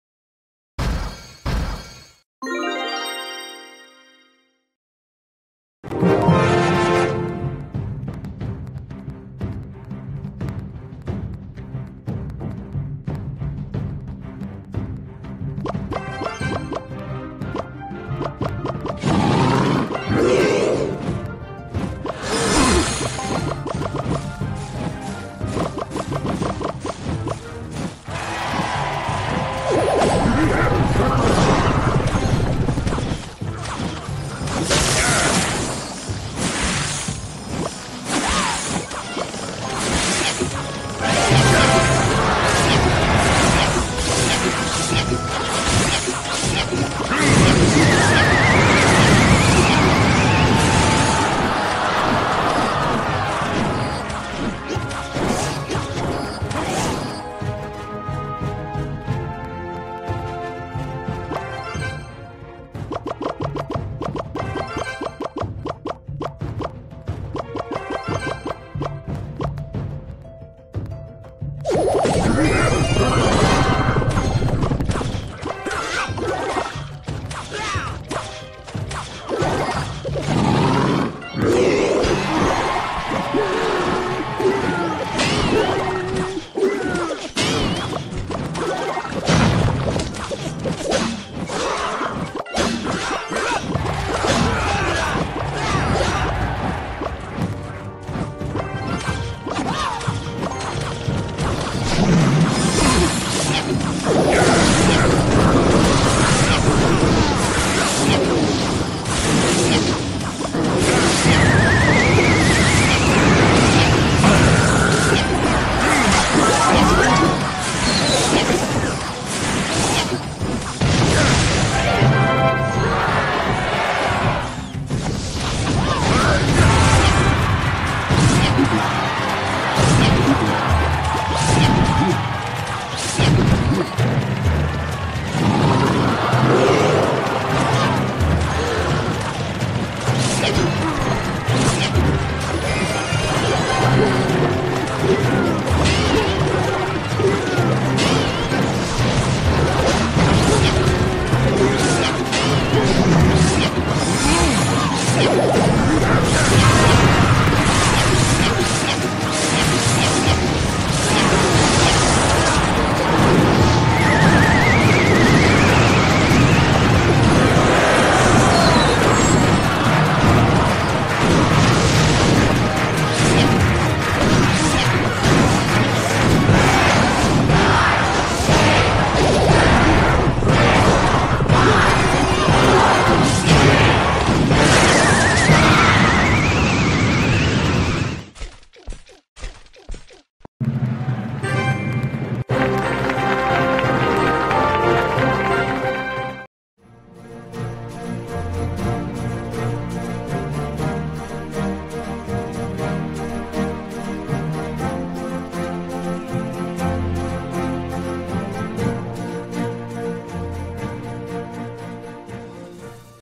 Ymgo❤Lava vs 건휘왔당 [TOP 200]
deck:
Lava Hound, Balloon, Fireball, Tombstone, Minions, Mega Minion, Barbarians, Zap

deck:
Mega Knight, Ice Wizard, Tornado, Barbarian Barrel, Fireball, Mega Minion, Royal Giant, Skeletons